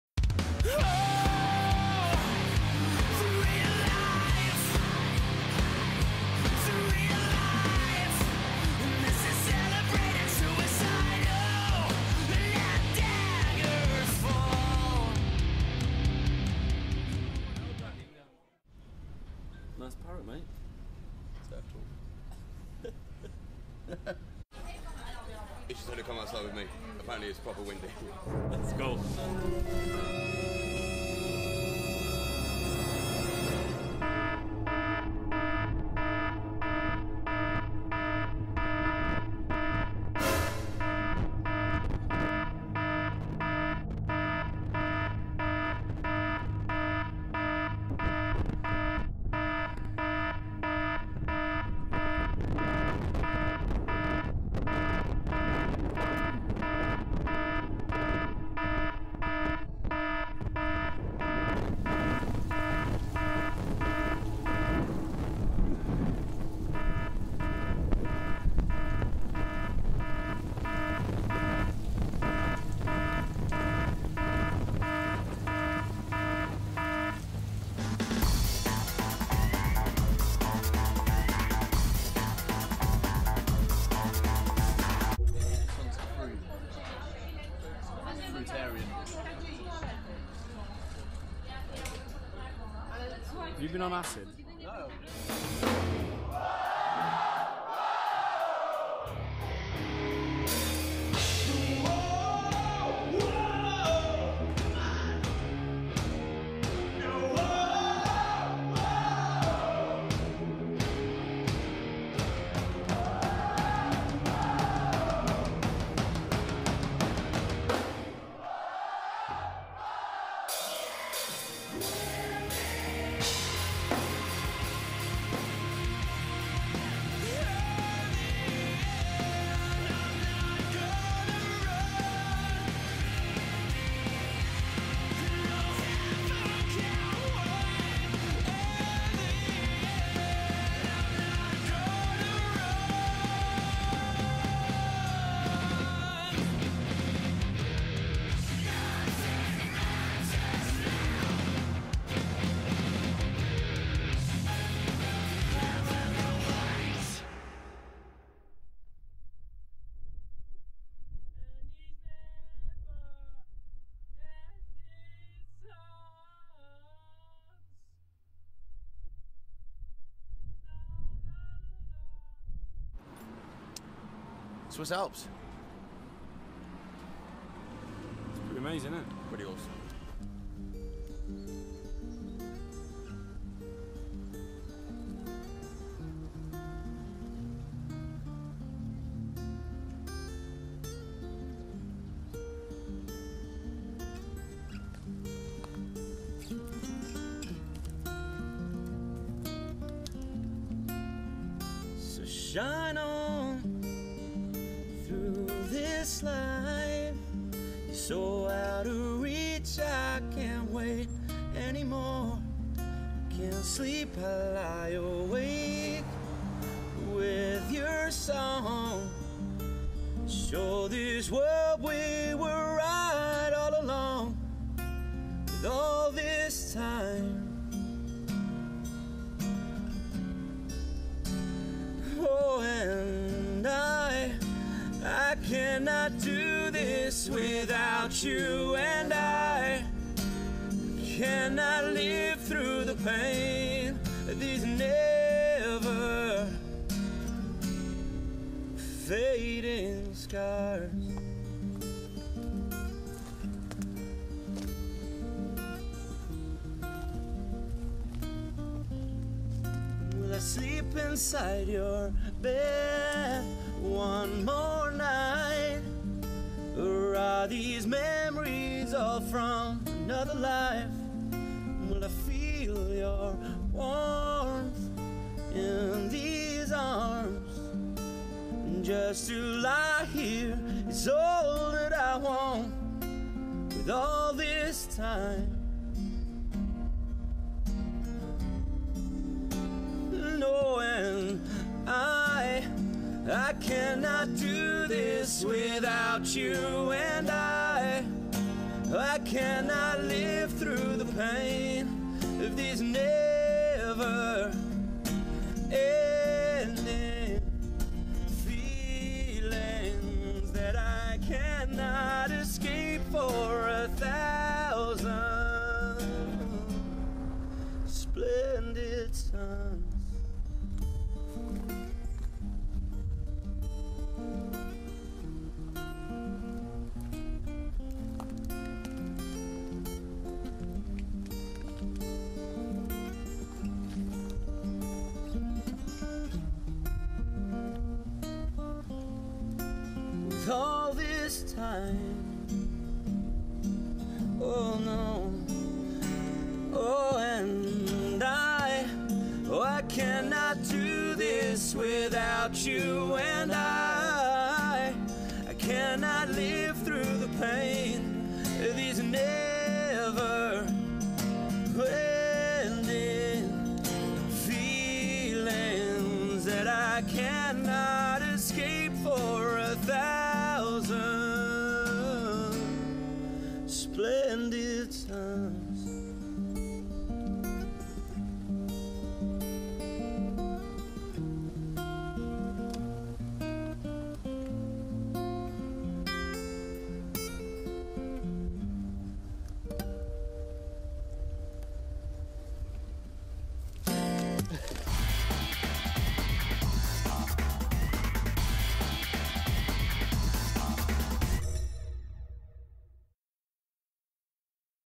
ZICO ON THE ROAD WITH HALESTORM 2012.mov
Some video footage of the Halestorm + Heavens Basement + Zico Chain European tour so far.